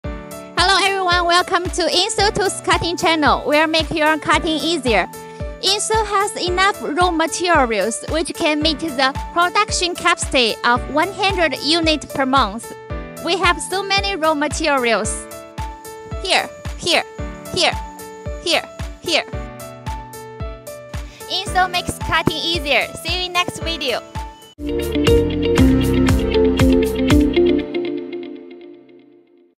Ensolltools Cutting Channel: Raw Material Display
where making cutting easier.Ensoll has enough raw materials whice can meet theproduction capacity of 100 units equipment per month.
We have so many raw  materials